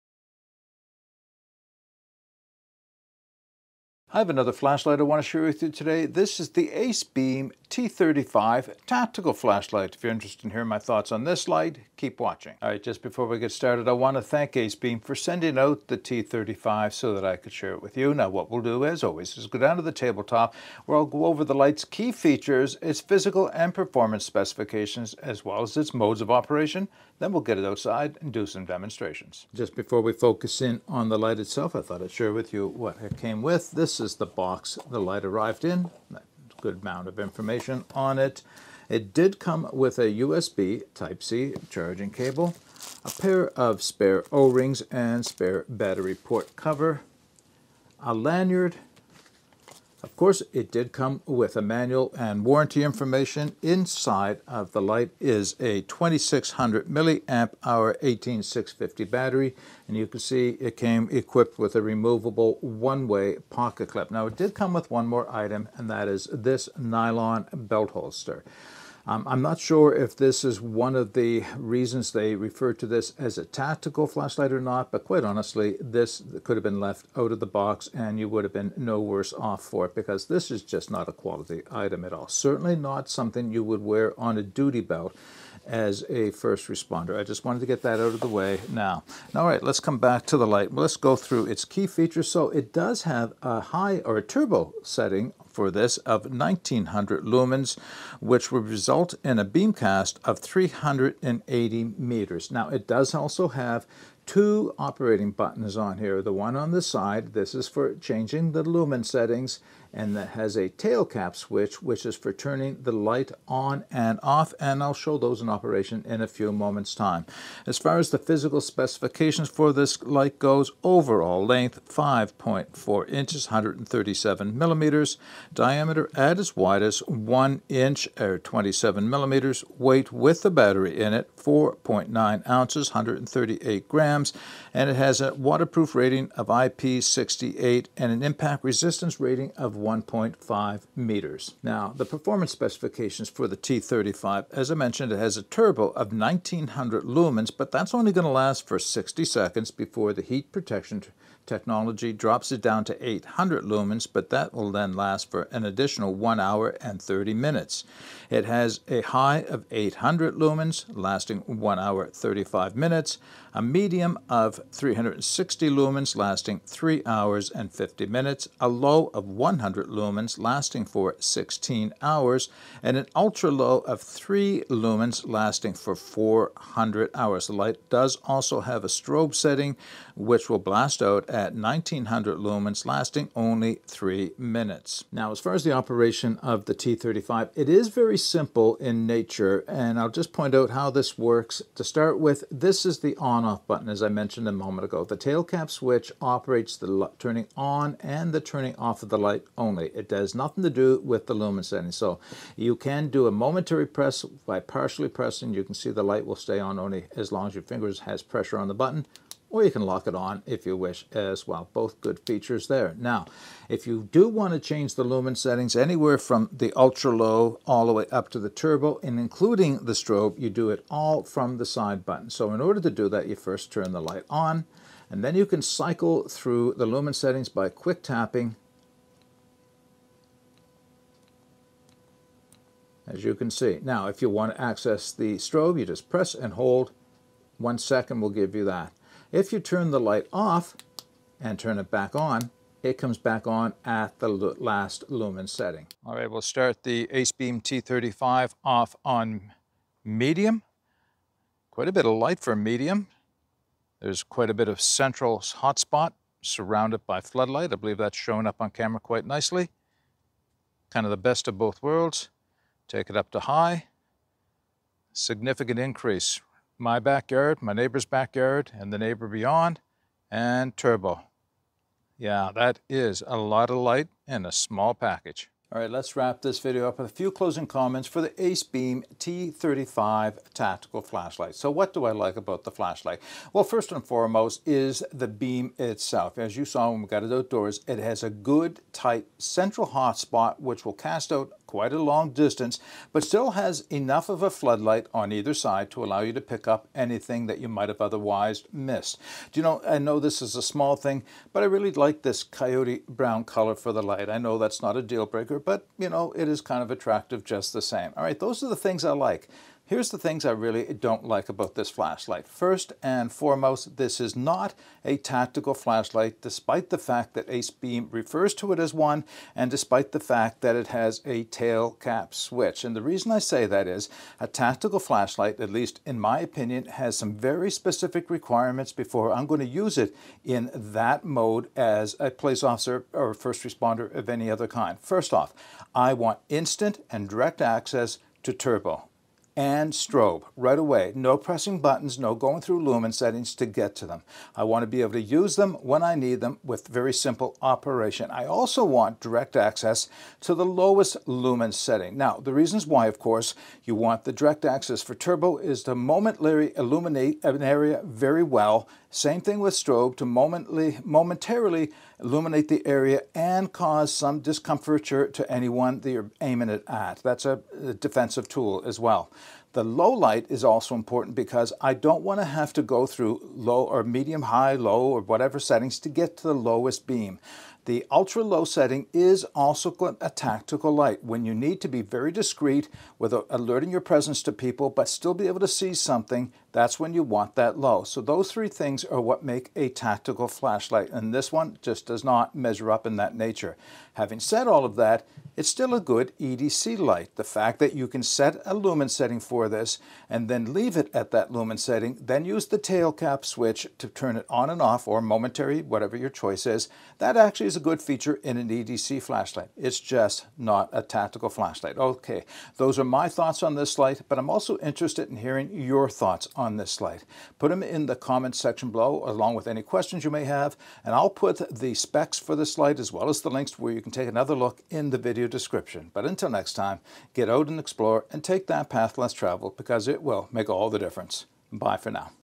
Acebeam T35 Tactical Flashlight
ACEBEAM T35
$64.0USD

Comes With
Belt holster
Lanyard
Pocket clip
USB-C cable
2xO-rings, charge port cover
2600mAh 18650 battery
Key Features
1900 lumens on turbo
380 meters beam cast
Dual operating buttons
Physical Specs
Waterproof  IP68
Impact   1.5m

Performance Specs
Turbo   1900L – 800L 60s – 1h30mins
High   800L   1h35m
Medium  360L   3h50mins
Low   100L   16h
Ultra-low  3L   400h
Strobe   1900L  3mins